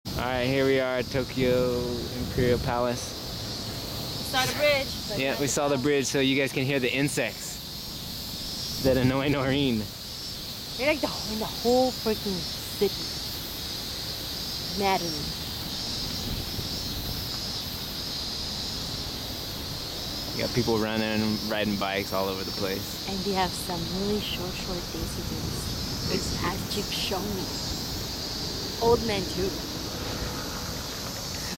Tokyo Imperial Palace cicadas
Nohreen got so annoyed with this little guys. They can be really loud sometimes. I enjoyed it though.